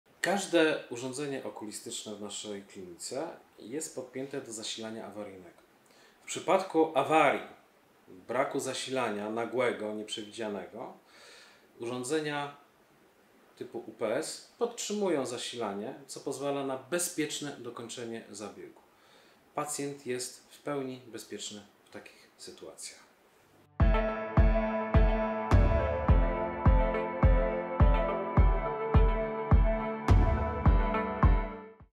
Co gdyby zabrakło prądu podczas laserowej korekcji wzroku? | SwissLaser Warszawa
Decydując się na zabieg laserowej korekcji wzroku pacjent może zadawać sobie liczne pytania dotyczące jego bezpieczeństwa w trakcie zabiegu. Jednym z nich jest to, co mogłoby się stać, gdyby na sali operacyjnej zabrakło prądu. O procedurach bezpieczeństwa związanych z laserową korekcją wzroku dowiesz się właśnie z tego materiału.

Inne ważne pytania zadawane przez pacjentów:

Co należy zmienić w codziennej rutynie po laserowej korekcji wzroku?